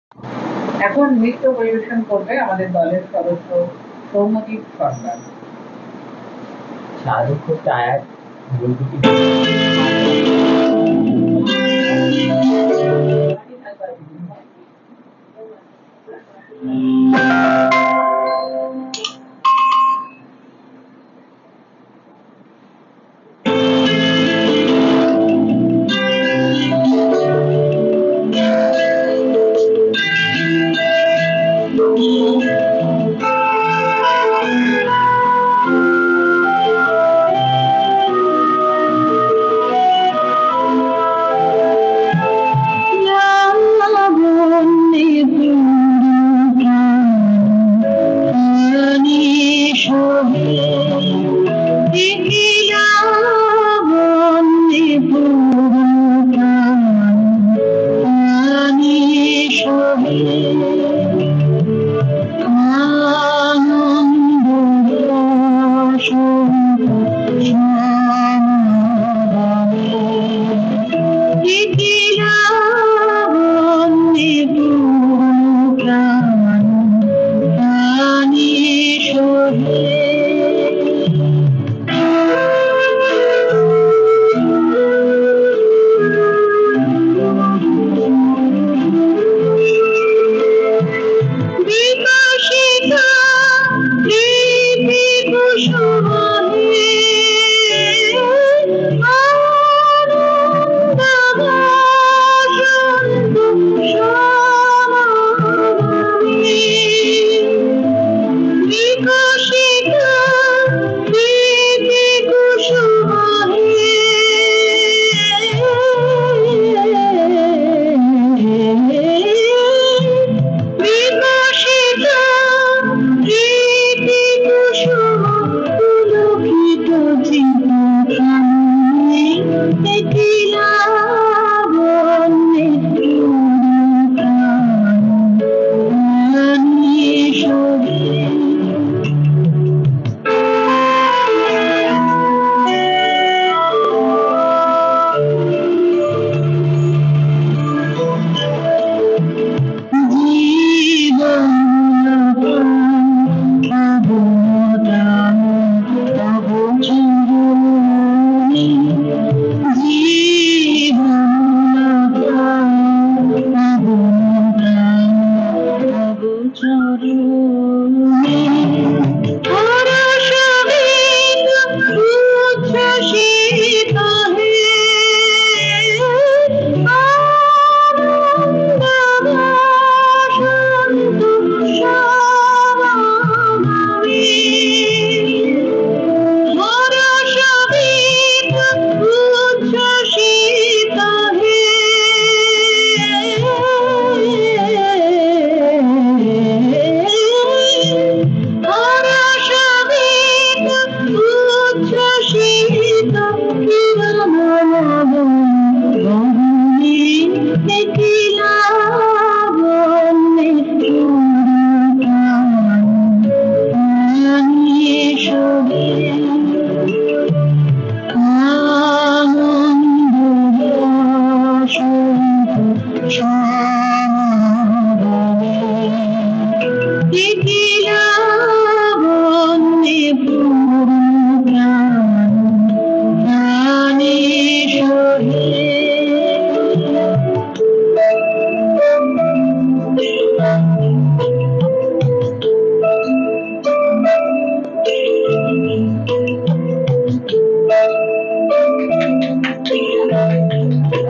নৃত্য পরিবেশনা || Dance Performance ★ সৌম্যদীপ || Soumyadeep ★ শ্রাবণ বরিষণ ১৪৩০ Rabindra Nritya
A Celebration of the Bengali New Year organised by, Naihati
Dance Performance : Soumyadeep Sarkar
Presented by Naihati Rangasena
Participated by all members of Naihati Rangasena and Nanhasaa
Offline presented on 8th August , 2023 at Lohaghat, Naihati

Officeal Instagram : @naihatirangasena

Officeal Instagram : @nanhasaa

Other performances are coming soon